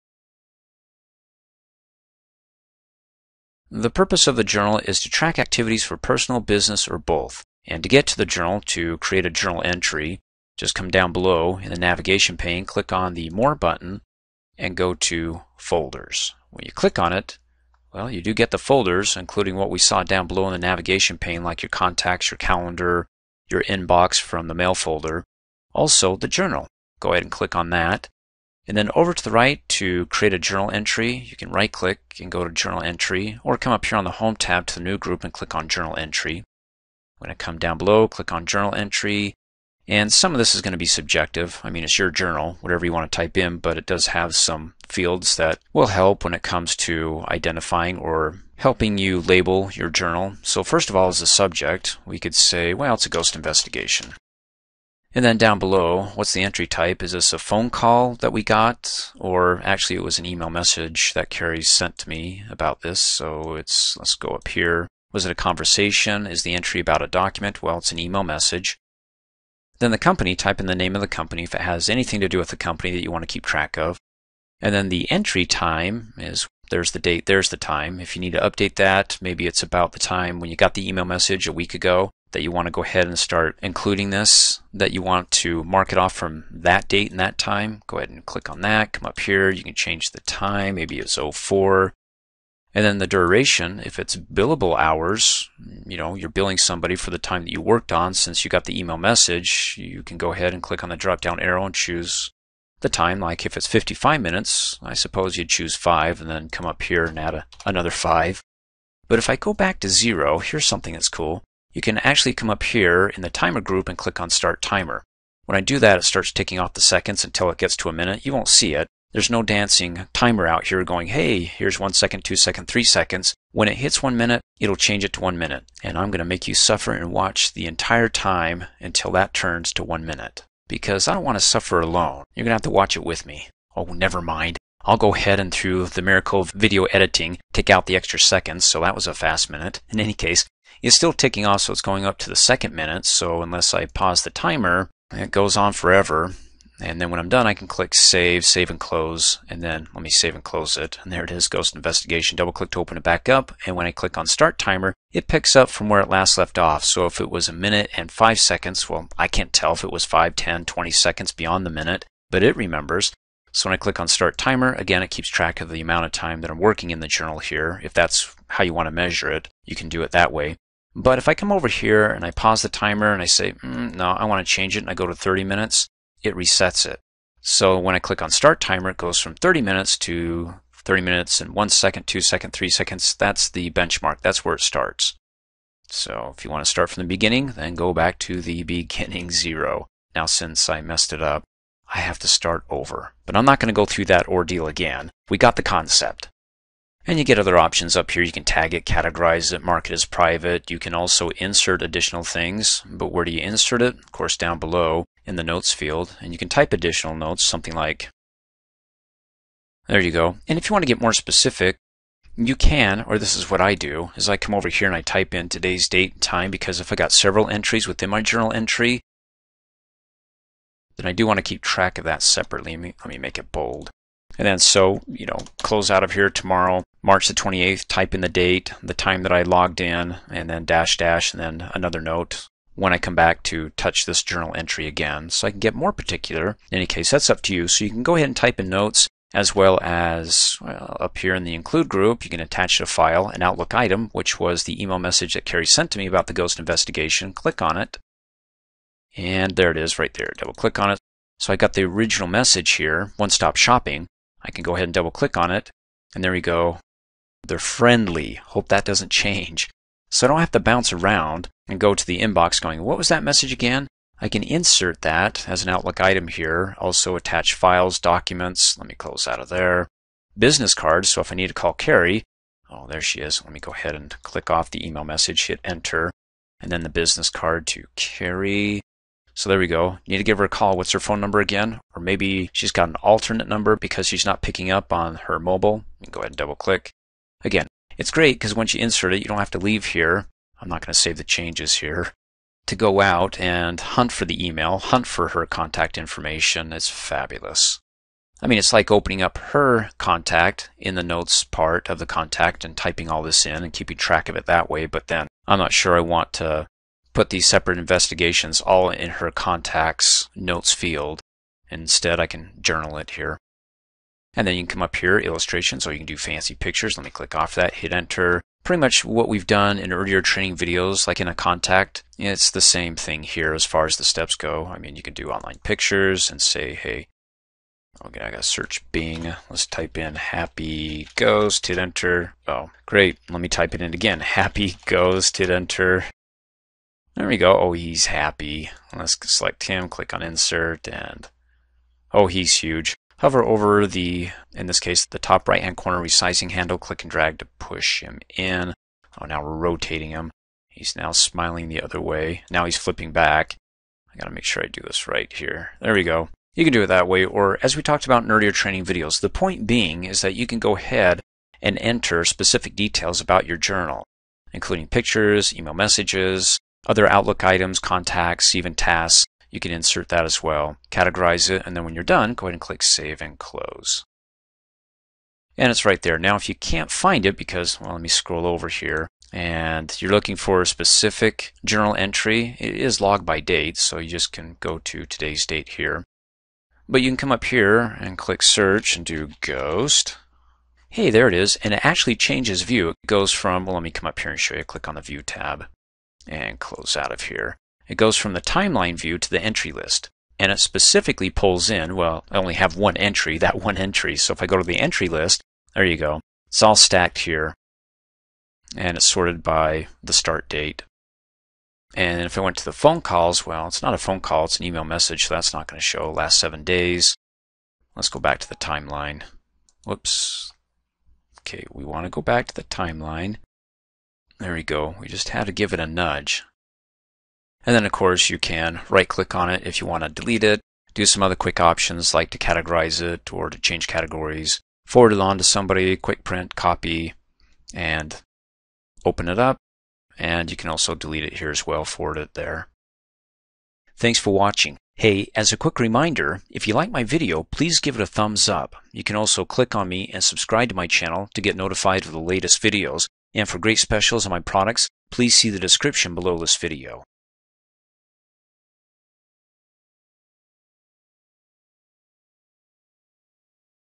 Microsoft Outlook 2016: Journal Entries
Microsoft Outlook 2016 training video on how to use the Journal feature in Outlook including Journal entries for: tracking activities which can be entered and recorded, and other activities like email messages, meeting info, task requests all for a specific contact, and also Office applications, and display them in a variety of timeline views like on a daily, weekly or monthly basis.